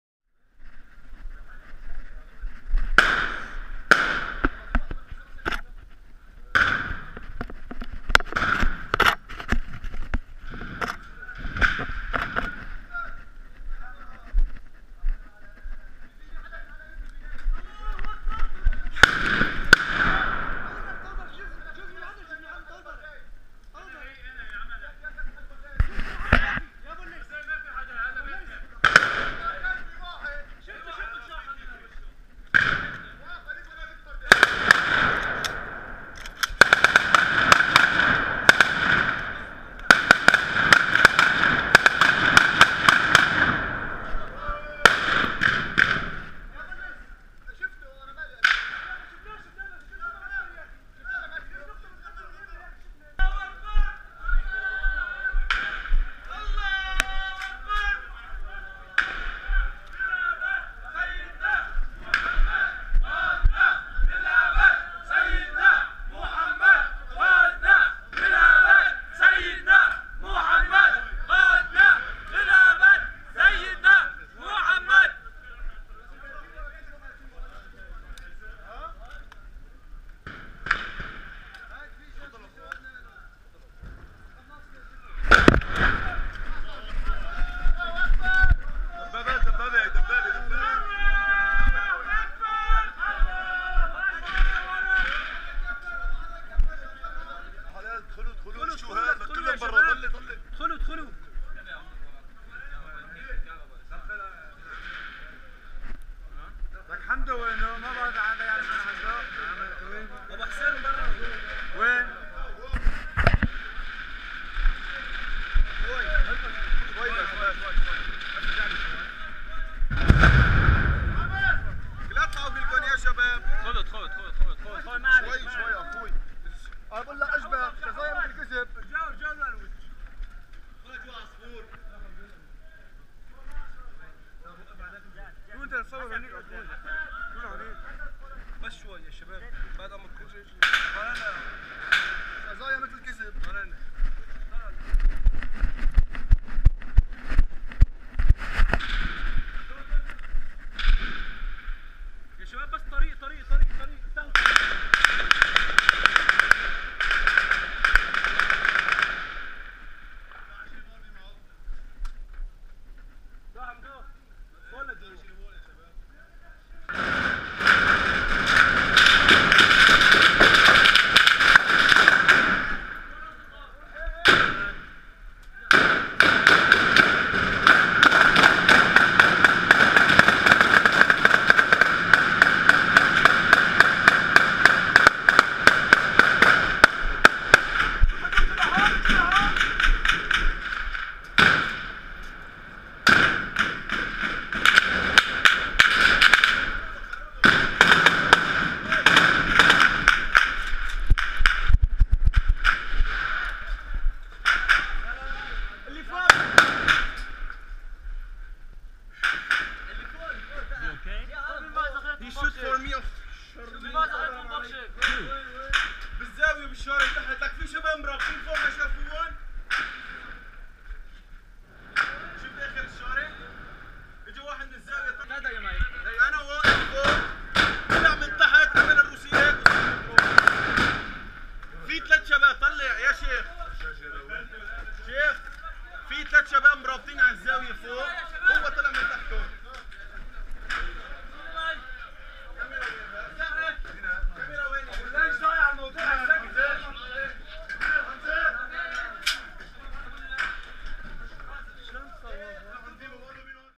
Guerra na Syria
Vindo de um intenso confronto na cidade de hama na siria.